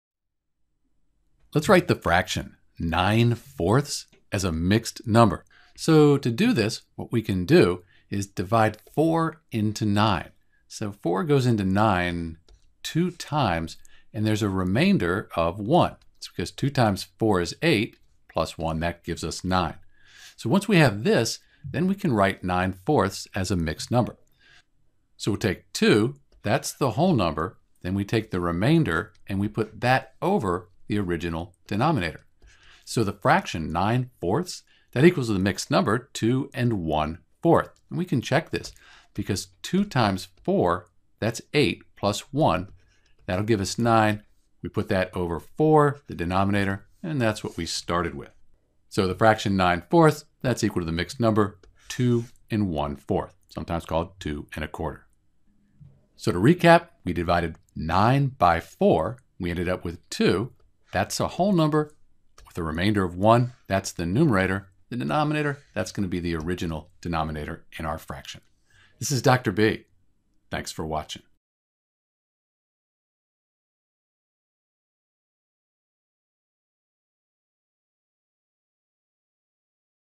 Write 9/4 as a Mixed Number
In order to write 9/4 as a mixed number in simplest form (sometimes people will say "as a whole number"  or "mixed fraction" ) we can follow a few steps.

Note that 9/4 is already simplified (in lowest terms). So we can't reduce or simplify 9/4 any further.  But we can write is as a mixed number.

First we can divide the numerator (9) by the denominator (4).  4 goes into 9 a total of 2 times with a remainder of 1.

Second, we take the remainder and put it over the original denominator.  This gives us 1/4.

For the mixed number we have the whole number 2 and the fraction (from the remainder) of 1/4.

So 9/4 written as a mixed number is 2 1/4 (two and one-forth).

To check your work you can now multiply the whole number in mixed fraction by the denominator (2 x 4) and add this to the numerator (the 1 in the1/4) you will get 9/4.  This is a good way to check that you have the right mixed fraction.